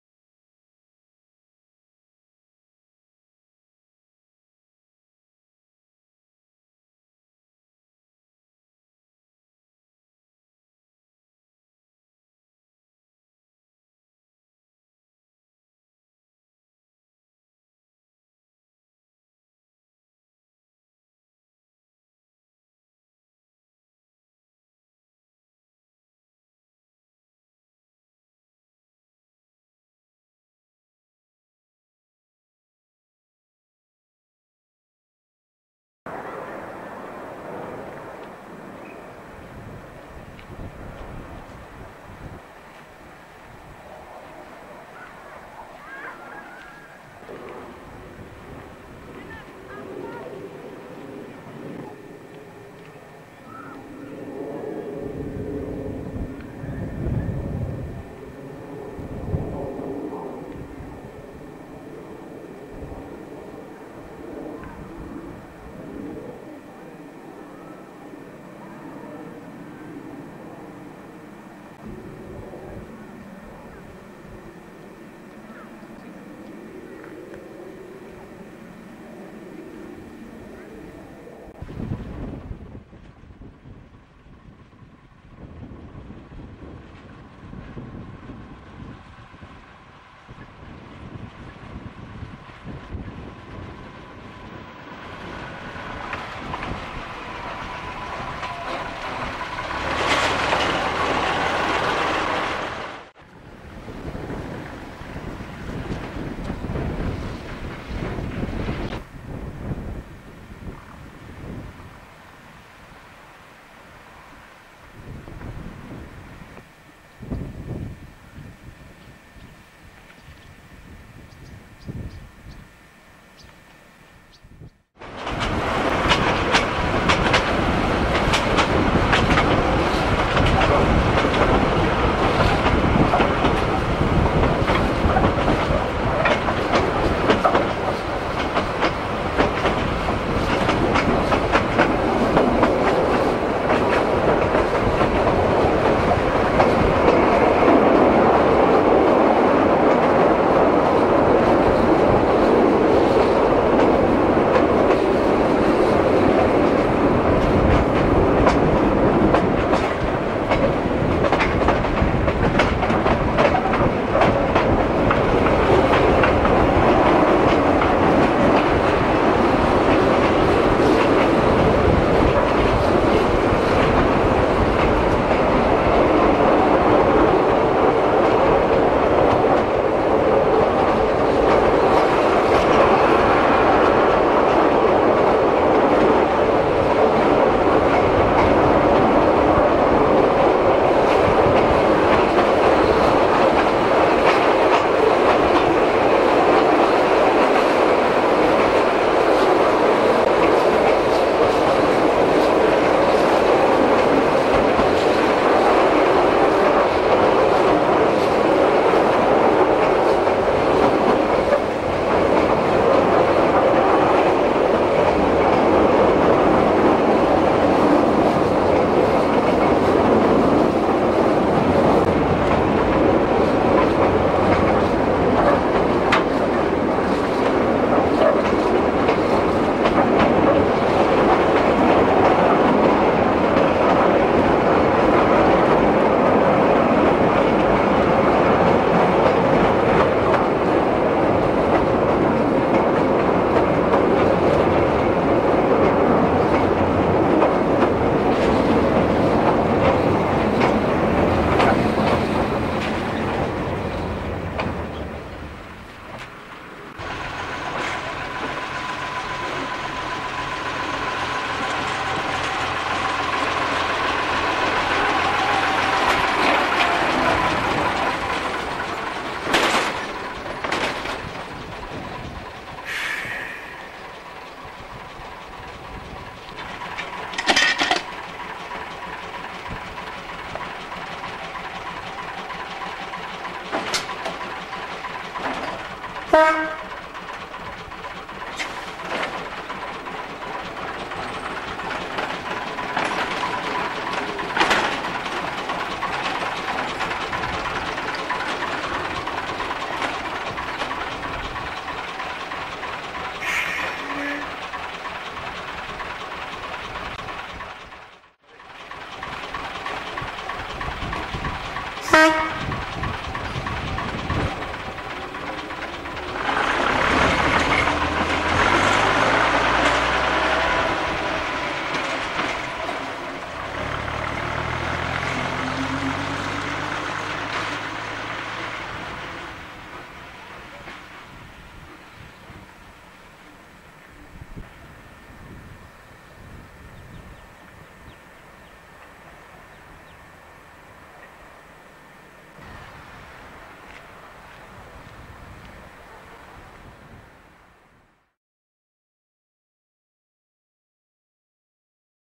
WELSH HIGHLAND HERITAGE RAILWAY 1997
A QUICK LOOK AT THE WHR STILL IN COURSE OF CONSTRUCTION, LATER TO BECOME A 25 MILE LONG RAILWAY, AND ALSO AT THE SHORTER WELSH HIGHLAND HERITAGE RAILWAY.